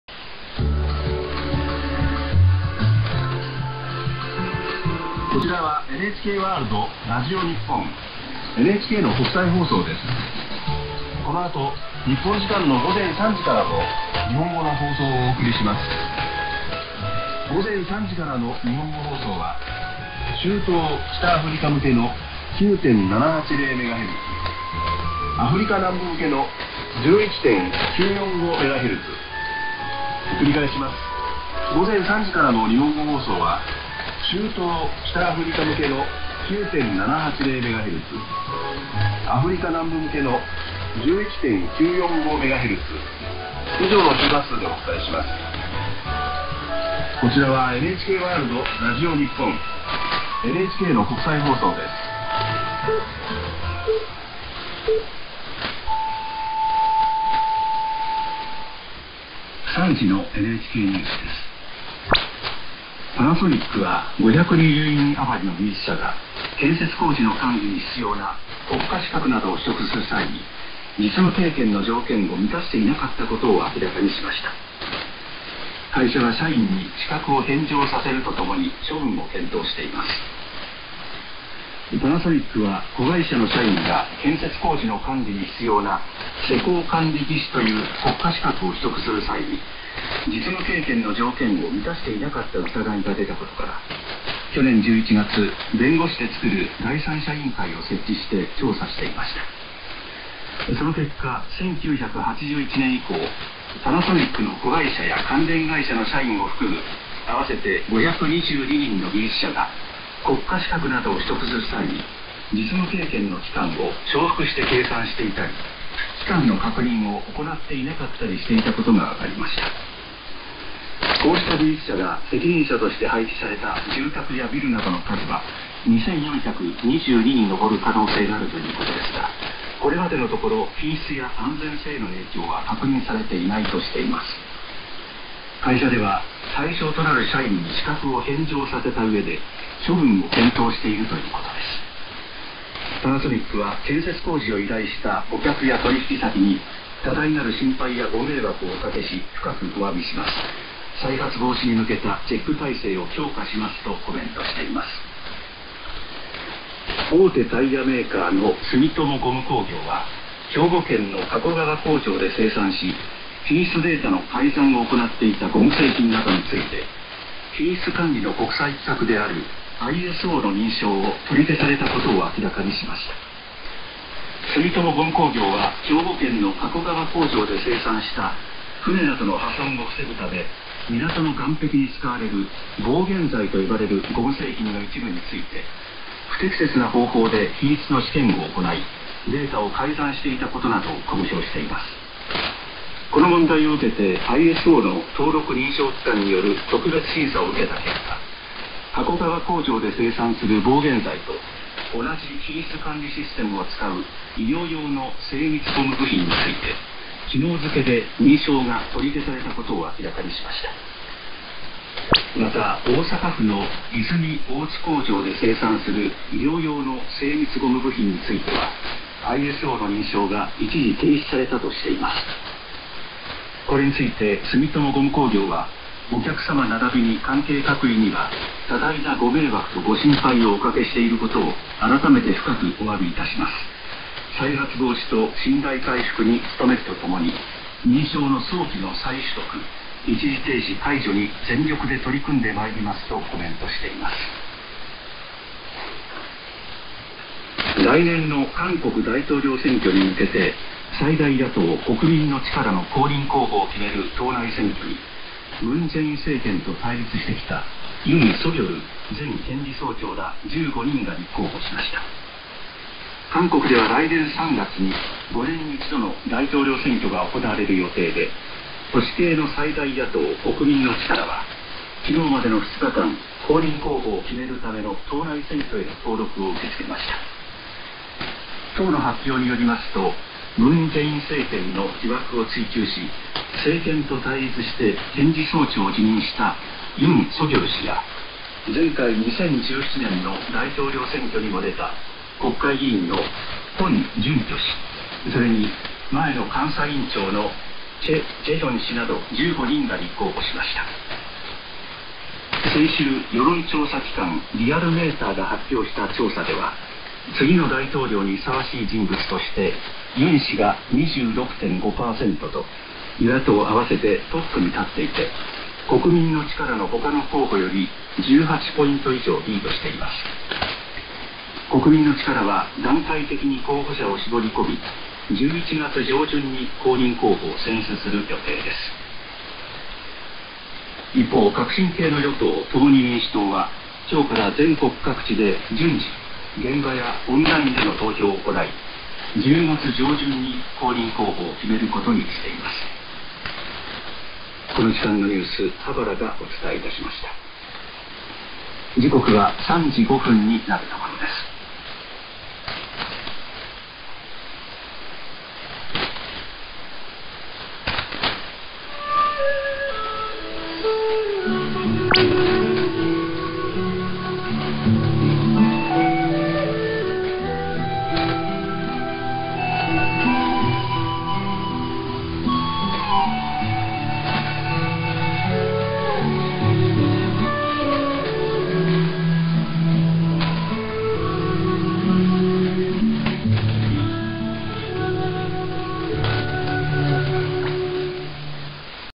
NHK World Radio (presumed) received in London, UK
Recorded outdoors on 9780 kHz in London, UK on 31/08/21 at 1759 UTC with GPD MicroPC, AirSpy Mini + SpyVerter 2 and Wellbrook UMB130 balun / 2x6m dipole.